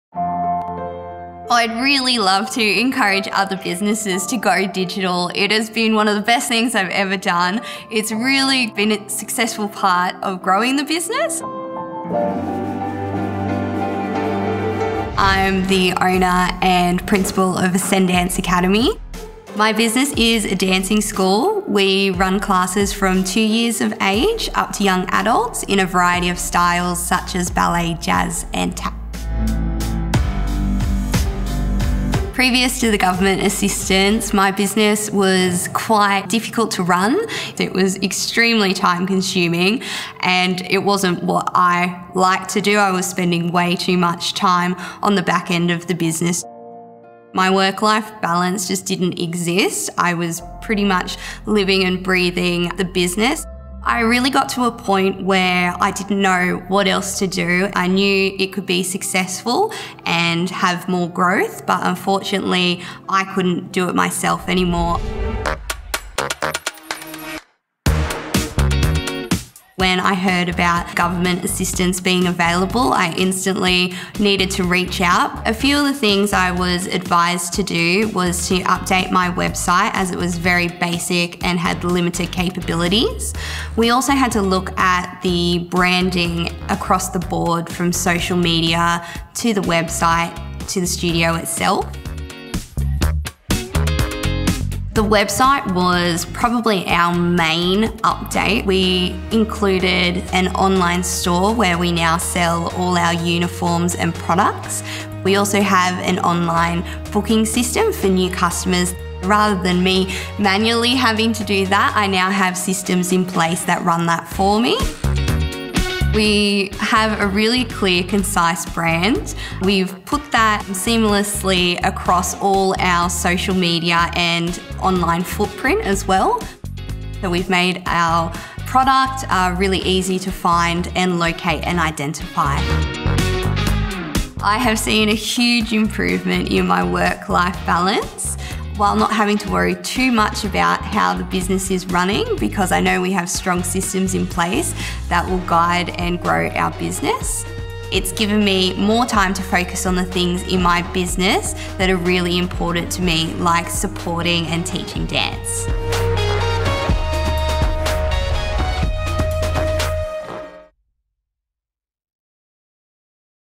Ascendance Academy - Digital Case Study
Ascendance Academy is a dance studio where creativity, individuality and self-expression are encouraged.

With the help of government assistance, Ascendance Academy implemented digital solutions which reduced admin time by 25 hours per week and increased enrolments by 58%.